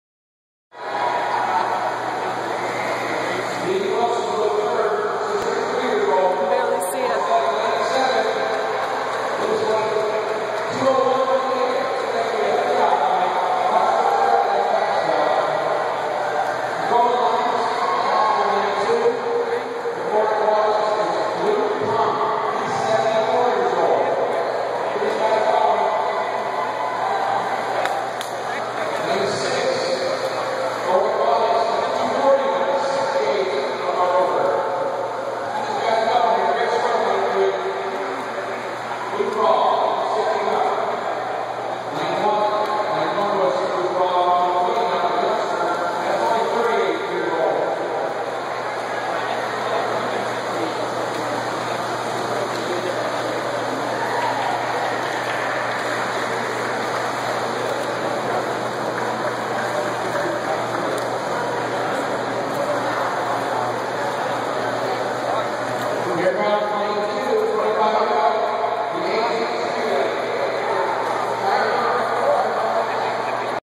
Debut flight for WET air swimmer marketing shark.mp4
The WET (Water Exercise Technologies shark makes its debut flight at the Rowdy Gaines Classic masters swim meet at the YMCA Aquatic Center in Orlando Florida